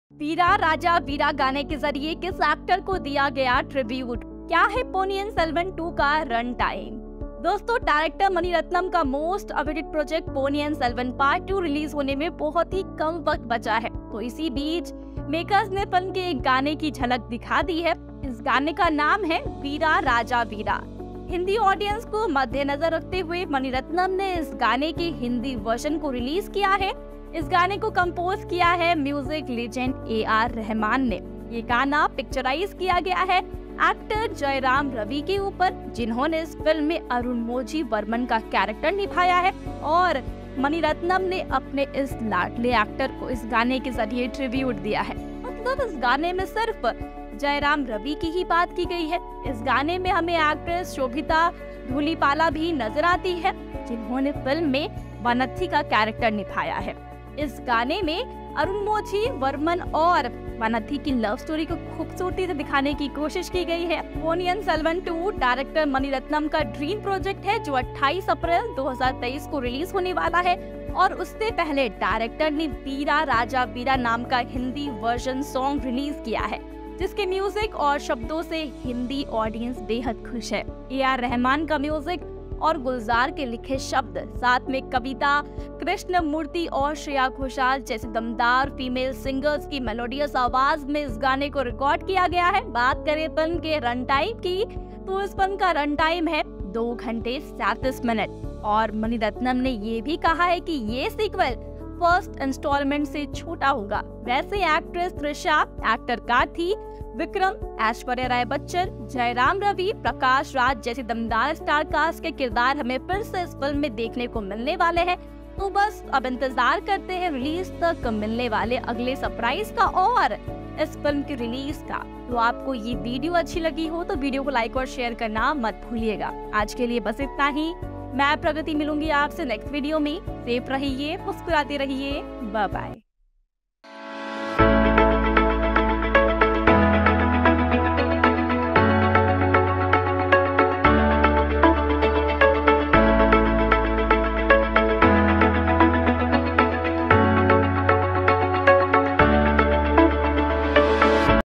Ponniyin Selvan 2| 35 Interesting Facts | Aishwarya Rai | Vikram | Jayam Ravi | Karthi | Mani Ratnam
Recent News- 'Ponniyin Selvan 2' run time revealed; the sequel will be shorter than the first part

0:00- intro
0:01- Kis gaane se kis actor ko diya gaya tribute ?
0:04- Kya hai Ponniyian Selvan 2 ka run time ?

Mani Ratnam's Magnum opus Ponniyin Selvan 2 is gearing up for a grand release next month. While fans are eagerly waiting with bated breath for the sequel after the blockbuster PS 1, the makers are all set to announce updates. Today, the makers shared a fun video of Ponniyin Selvan's cast enjoying on the sets.

The makers of the film took to Twitter and shared a BTS video of Ponniyin Selvan's cast speaking about PS 1 and PS 2. Jayam Ravi, Karthi, and Chiyaan Vikram are seen sharing experiences about their roles from the sets of the film. A few fun moments of the team from interviews with Ponniyin Selvan 1 are also included in the video.

Watch the BTS video of the team on sets of PS 1 and PS 2:
Ponniyin Selvan 2's updates
There were rumours a few days ago that the release of PS 2 was postponed. However, the team clarified and said that the sequel will be released as announced. Ponniyin Selvan 2 will release worldwide in theatres on April 28.

Another interesting update about PS 2 is the makers have reportedly finalised the trailer and audio launch date of the much-awaited project.  Like the first installment, the official trailer and audio of PS 2 will also get released on the same day, in a grand event that is slated to be held in Chennai, on April 5, this year. The makers are expected to confirm the trailer and audio launch date with an official announcement, very soon.

Ponniyin Selvan 2 Release date

About Ponniyin Selvan 2
Ponniyin Selvan is based on Kalki Krishnamurthy's 1955 novel of the same name. The film will feature the star-studded cast reprising their roles respectively such as Aishwarya Rai Bachchan as Nandini, Vikram as Aditya Karikalan, Karthi as Vanthiyathevan, Trisha as Kundavai, and Jayam Ravi as Arunmozhi Varman. In addition to this, Sarathkumar, Prabhu, Lal, Kishore, Ashwin Kakumanu, and Aishwarya Lekshmi are also part of PS 2.

Financed by Mani Ratnam and Allirajah Subaskaran under their banners Madras Talkies and Lyca Productions, music maestro AR Rahman has set fresh chartbusters after giving us blockbuster songs in PS 1.

 ponniyin selvan 2 book story ponniyin selvan 2 box office prediction

Anchor pragati
writer Trupti
Editor Pranit

Thumbnail: Mani Ratnam का tribute?